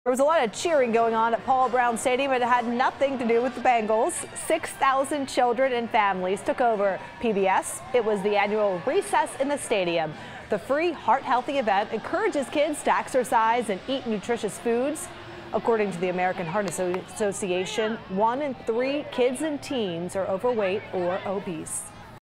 Children learn about exercise at Paul Brown Stadium
CINCINNATI (WKRC) – Families spent a fun day at Paul Brown Stadium and kids were able to learn about keeping their hearts healthy at the same time.

Six thousand children and their families took part in the annual “Recess in the Stadium” on Saturday.

The free heart healthy event encourages kids to exercise and eat nutritious foods.

According to the American Heart Association, one-in-three kids and teens are overweight or obese.